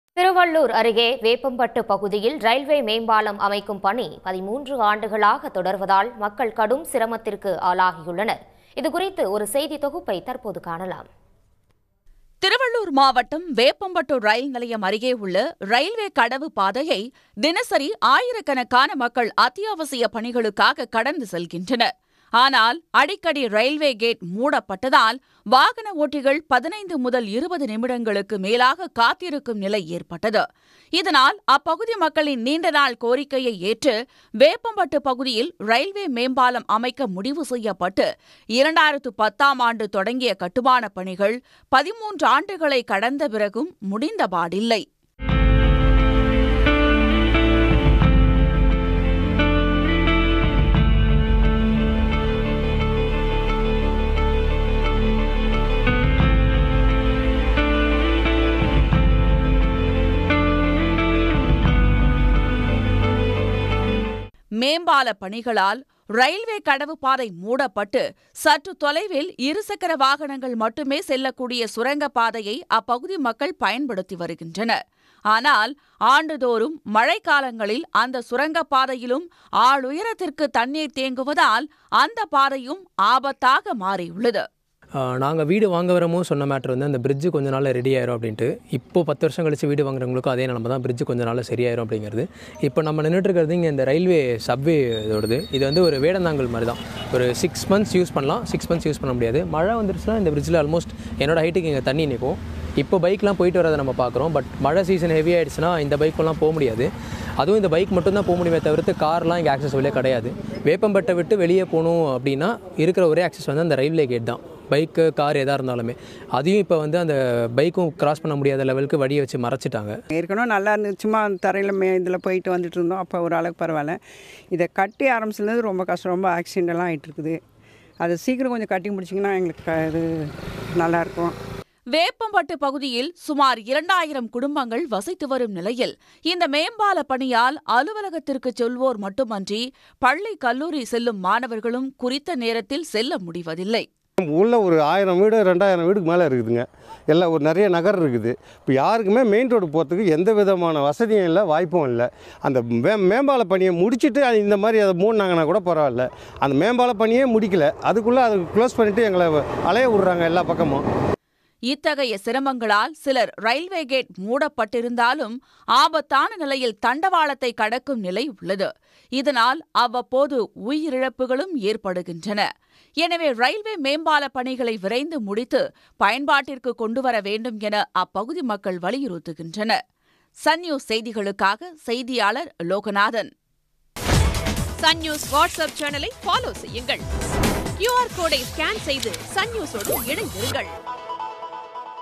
Veppampattu Rail Over Bridge Construction | Railway Station | Thiruvallur | Sun News
13 ஆண்டுகளாக தொடரும் மேம்பால பணி..
 விரைந்து முடிக்க பொதுமக்கள் கோரிக்கை